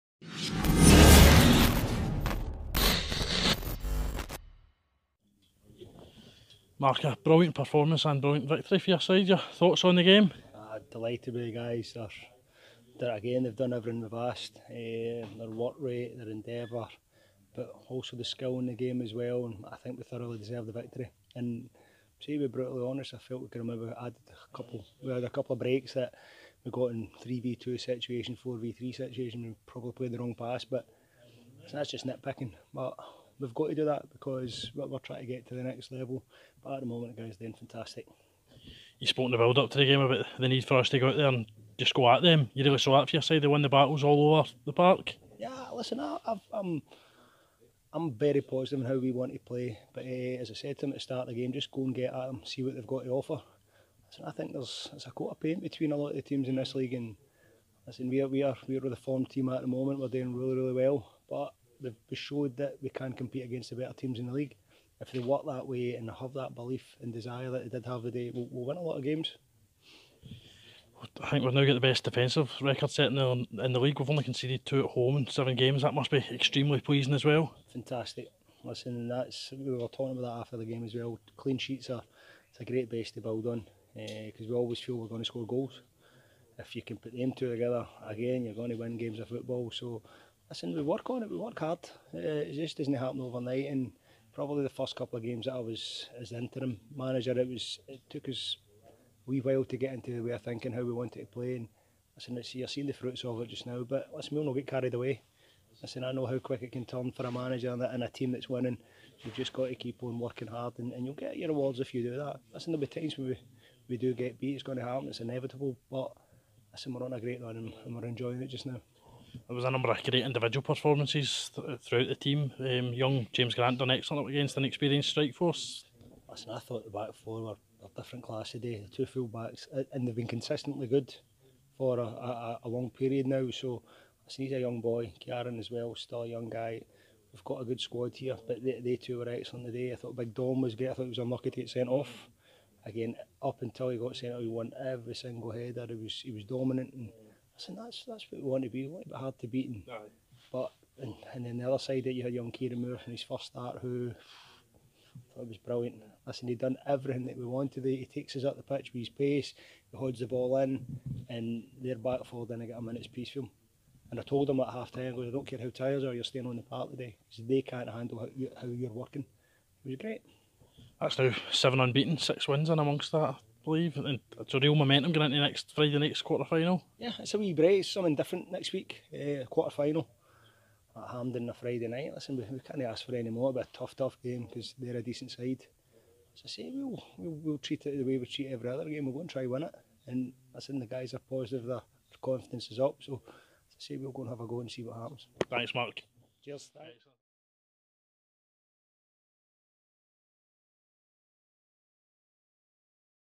Peterhead - 10 November 2018
Mark Roberts speaking to QPTV's Sean Davenport following the 2-0 home win over Peterhead.